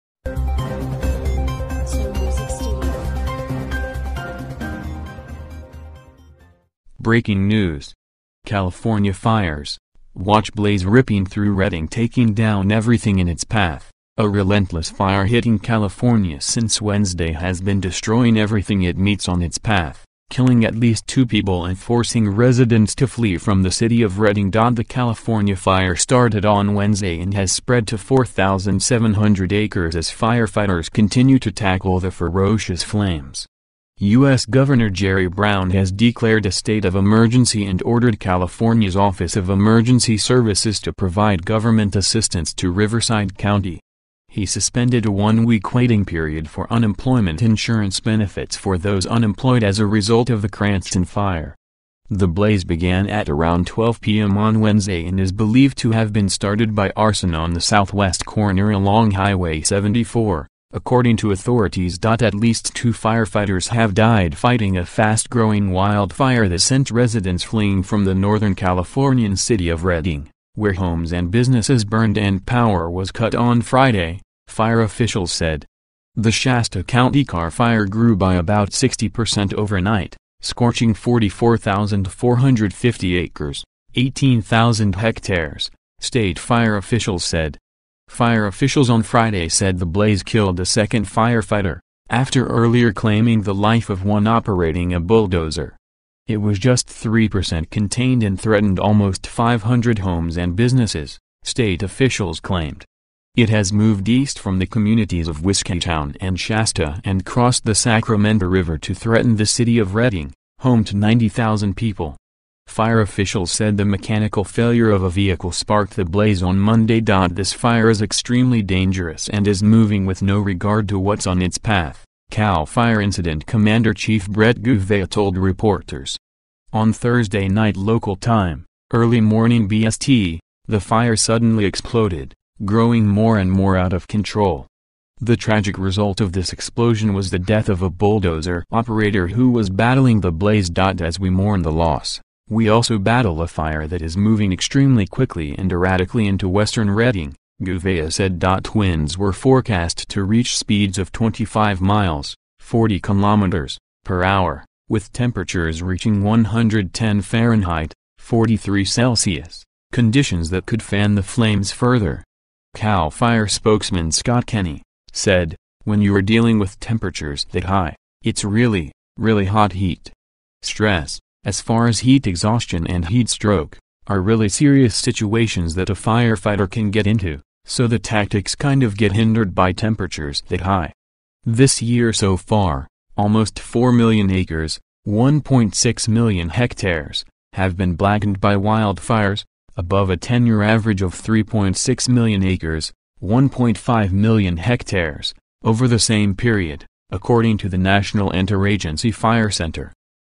BREAKING NEWS!!California fires: WATCH blaze ripping through Redding taking down everything in its p
California fires: WATCH blaze ripping through Redding taking down everything in its path